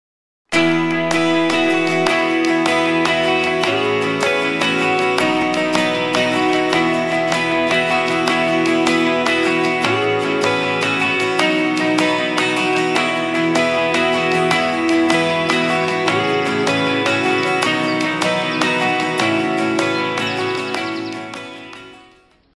Vamos à Cidade-Jardim, este fim-de-semana?
Um convite a um fim-de-semana em Viseu, onde ganham destaque algumas das paisagens do centro da cidade.